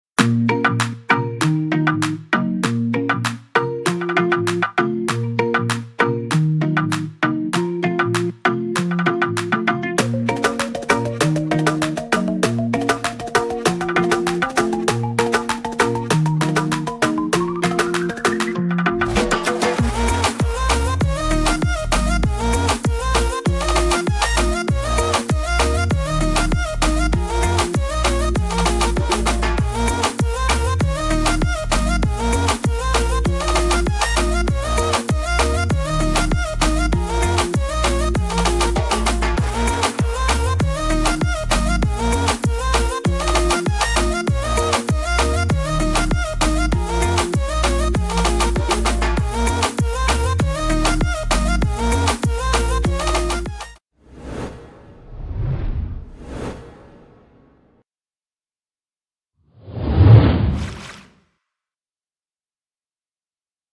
Le Caire se prépare pour la CAN !
Les rues du Caire sont prêtes à accueillir la Coupe D'Afrique des Nations ! 🇪🇬⚽🤩